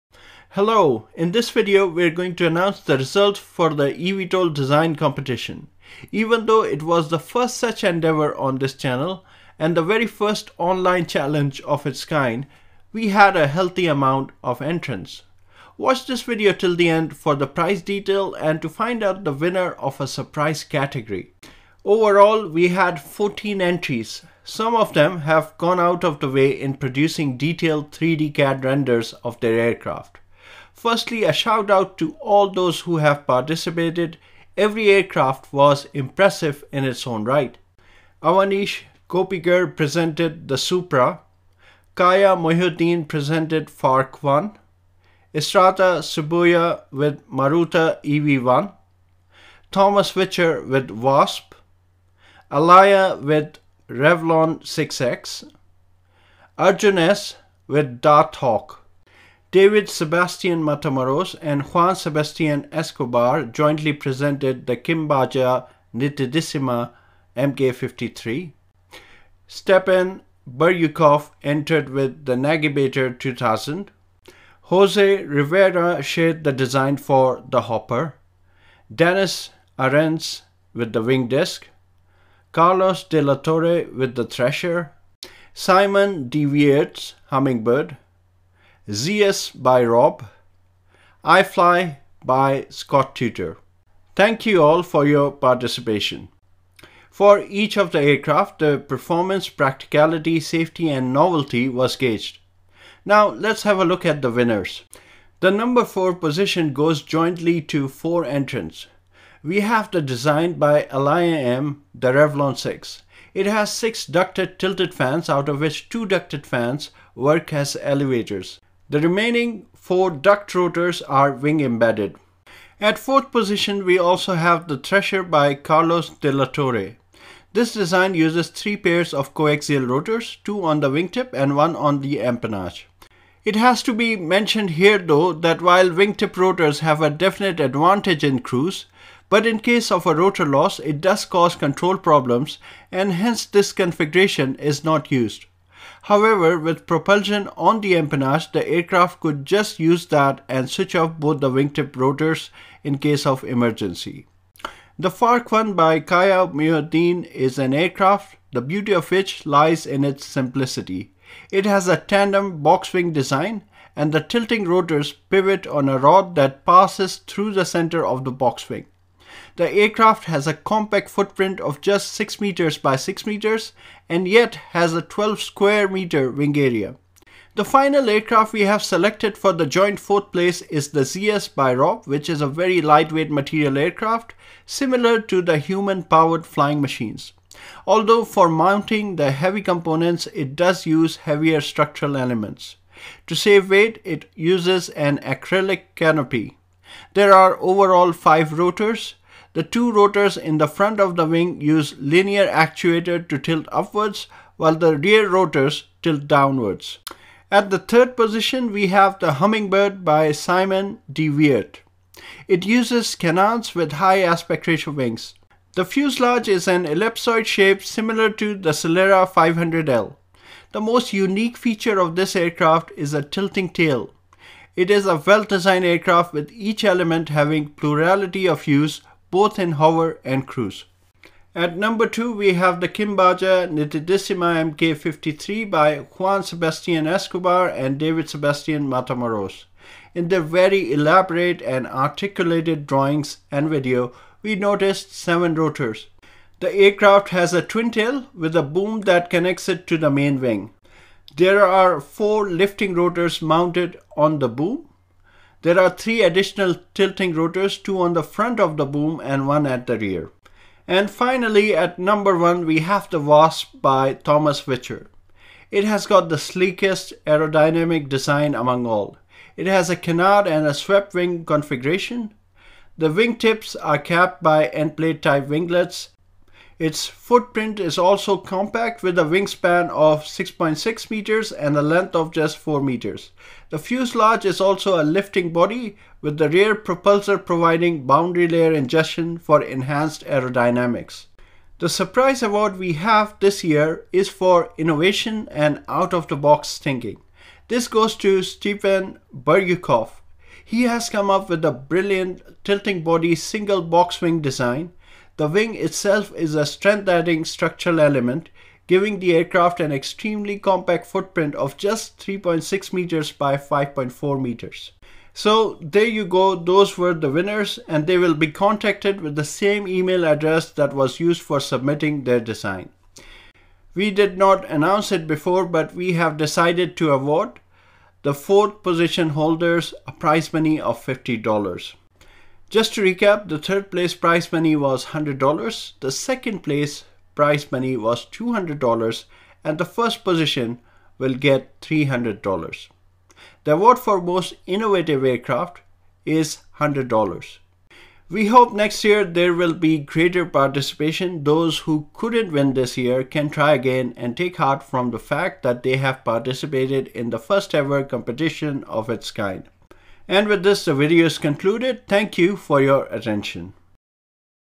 EVTOL Design Challenge 22 : The Best Designs
In this video we reveal the results of the first ever EVTOL design Challenge 2022

4th Place Winners
Elijah Mais
Carlos de La Torre
Kaja Mohieddin
Rob

3rd Place
Simon De Weerdt

2nd Place
Juan Sebastian Escobar and David Sebastian Matamoros

1st Place
Thomas Wiwchar

Innovation Award
Stepan Biryukov